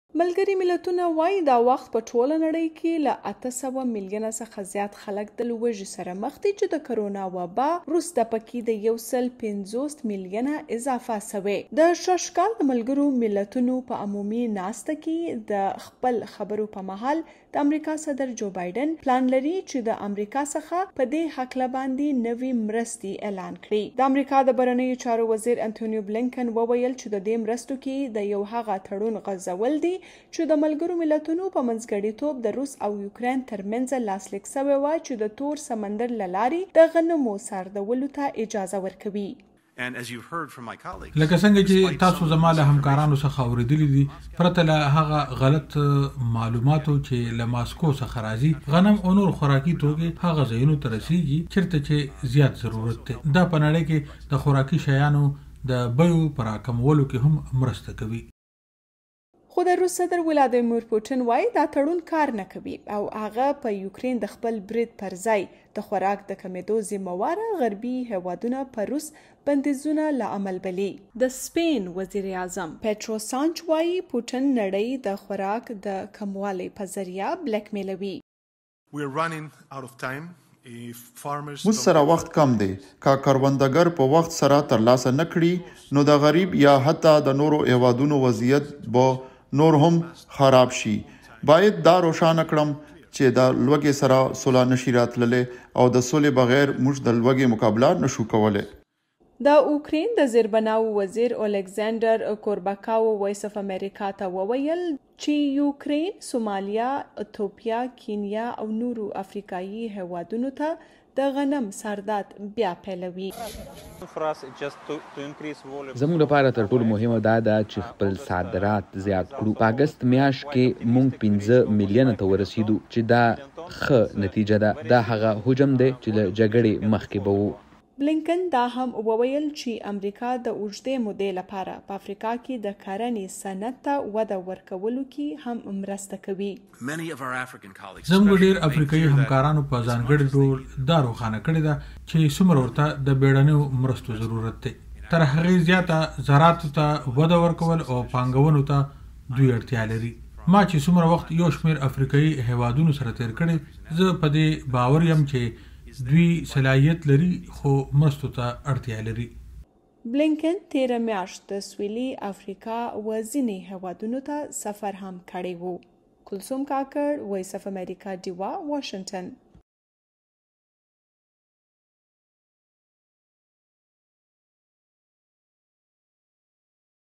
د متحده قومونو چارواکي: د وچکالۍ، ګرانۍ او د اوکراین د جنګ له امله ځیني ملکونه د قحط ښکار کېدلی شي
د متحده قومونو د جنرل اسمبلۍ د غونډې ترڅنګ د امریکې خارجه وزیر اېنټني بلېنکن به په نړیواله کچه د خوراک د تحفظ په موضوع د یوې غونډې کوربتوب کوي. دا په داسې حال کې د متحده قومونو چارواکي په یو شمېر ملکونو کې د قحط په اړه خبرداری کوي. په دې اړه سېنډي ساین رپورټ کلثوم کاکړ وړاندې کوي